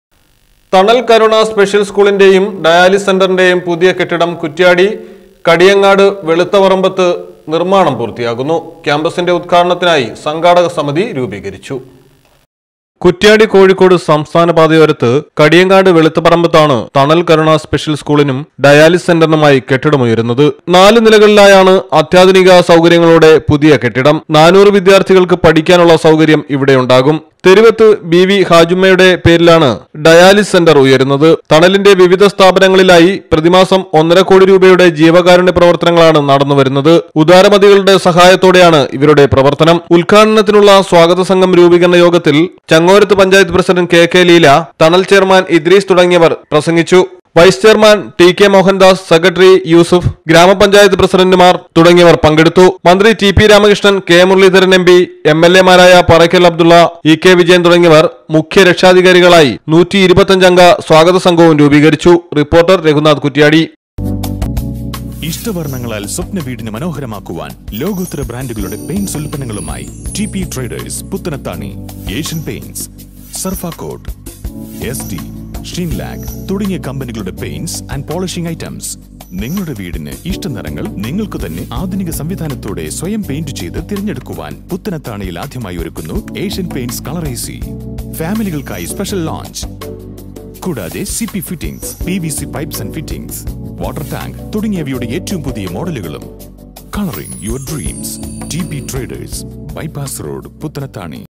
തണല്‍-കരുണ സ്‌പെഷ്യല്‍ സ്‌കൂളിന്റെയും ഡയാലിസിസ് സെന്ററിന്റെയും പുതിയ കെട്ടിടം
തണല്‍-കരുണ സ്‌പെഷ്യല്‍ സ്‌കൂളിന്റെയും  ഡയാലിസിസ് സെന്ററിന്റെയും പുതിയ കെട്ടിടം വെളുത്തപറമ്പത്ത് നിർമ്മാണം  പൂർത്തിയാകുന്നു